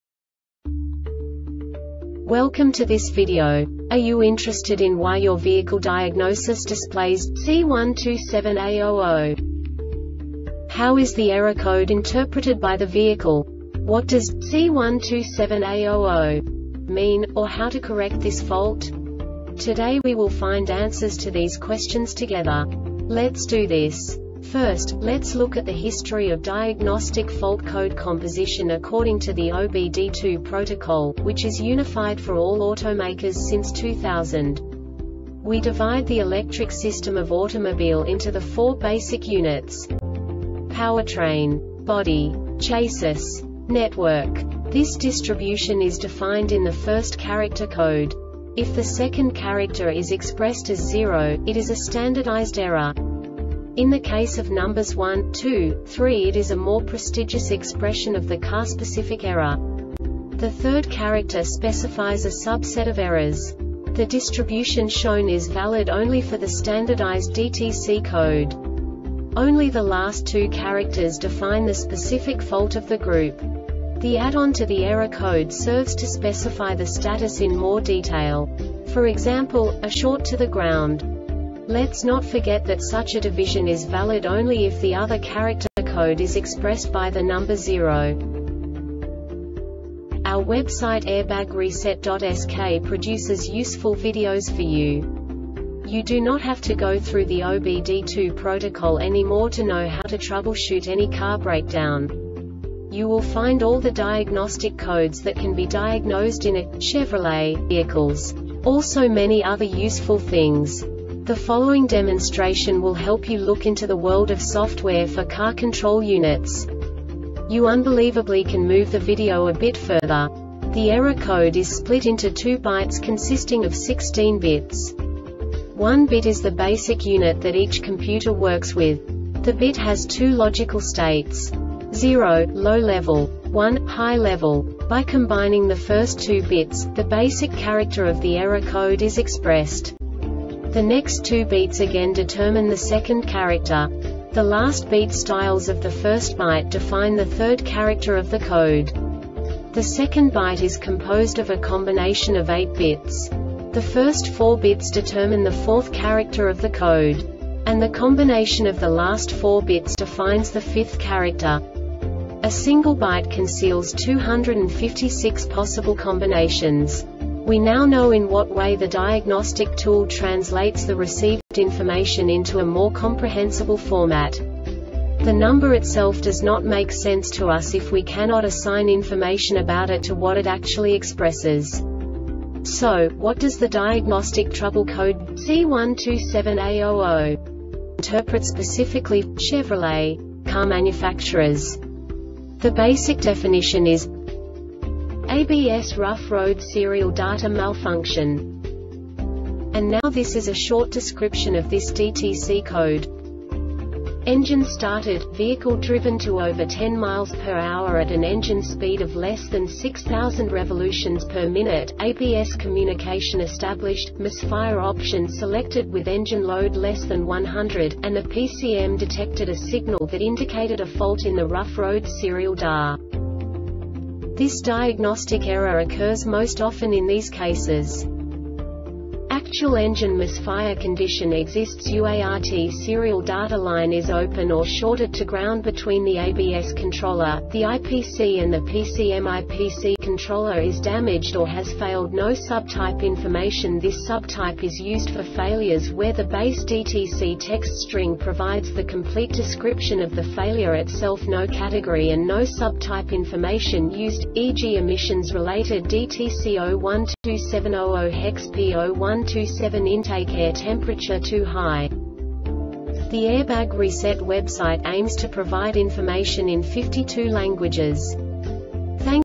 DTC Chevrolet C127A-00 Short Explanation
Contents:
0:21 Basic DTC analysis according to OBD2 protocol standard.
1:48 Insight into programming
2:58 A diagnostically understandable description of the Diagnostic Trouble Code
3:05 Basic error definition

Make:
Chevrolet
Code:
C127A 00
Definition:
ABS Rough Road Serial Data Malfunction
Description:
Engine started,vehicle driven to over 10 mph at an engine speed of less than 6000 rpm,ABS communication established,misfire option selected with engine load less than 100, and the PCM detected a signal that indicated a fault in the rough road serial da
Cause:
Actual engine misfire condition existsUART serial data line is open or shorted to ground between the ABS controller,the IPC and the PCMIPC (controller) is damaged or has failed
Failure Type: